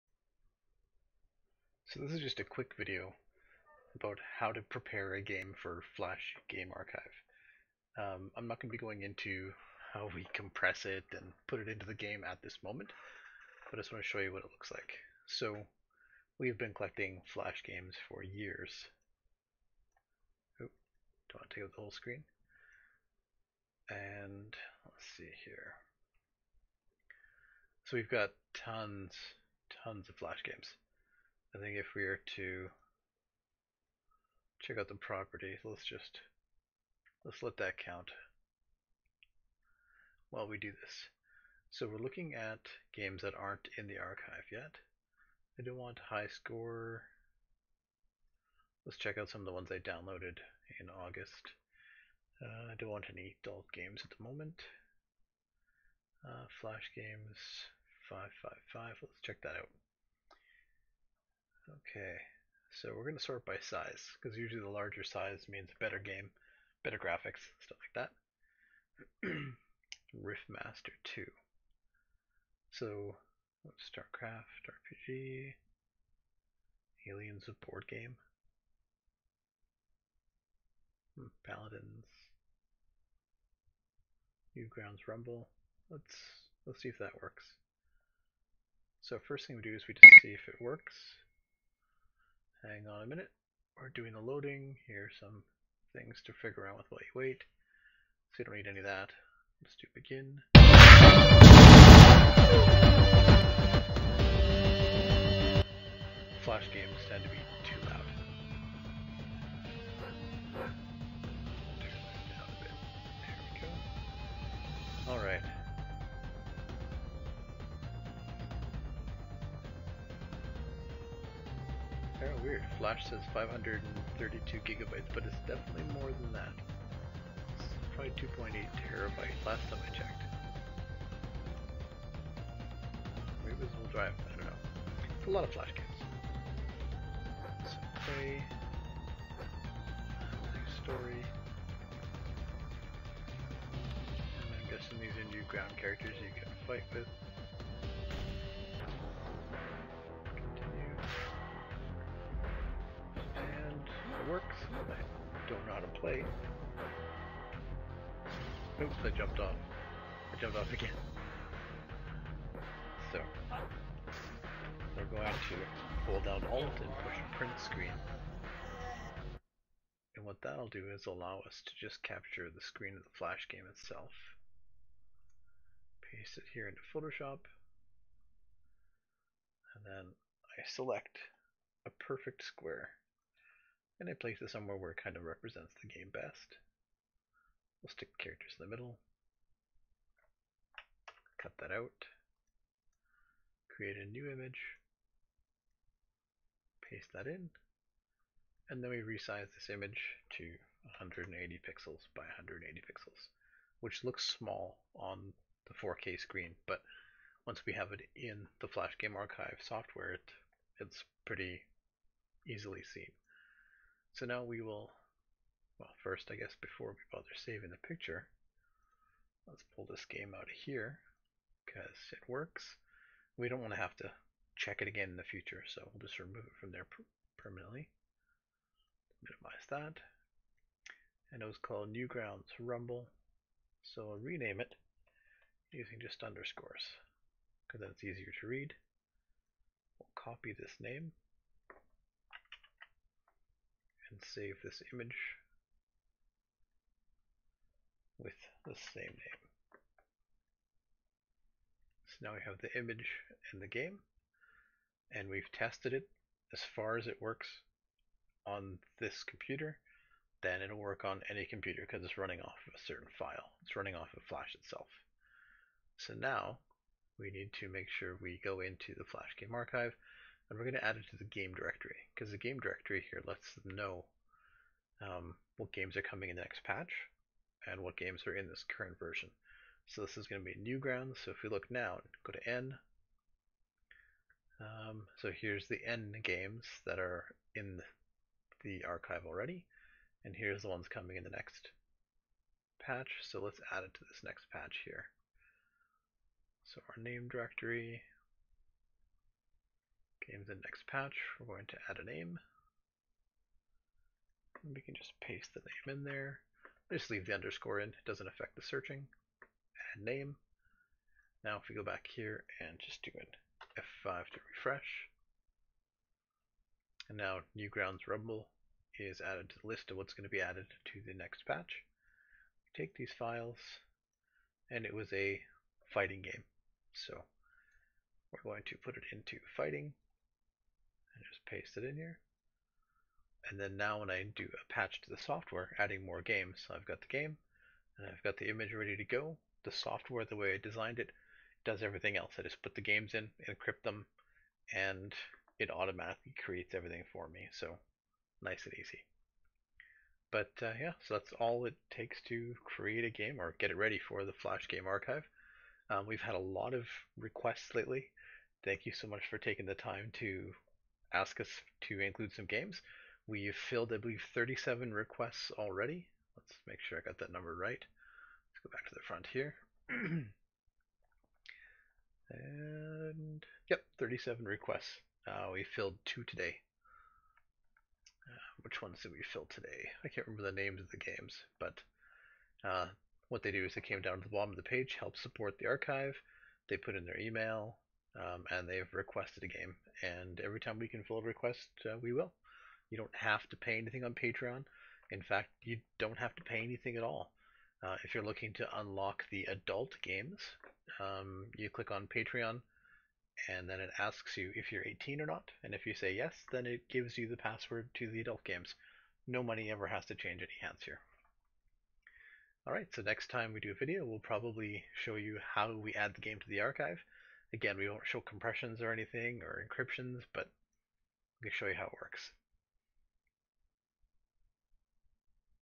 Flash Game Archive - Preparing a Game
A quick video about preparing a game for the Flash Game Archive Software.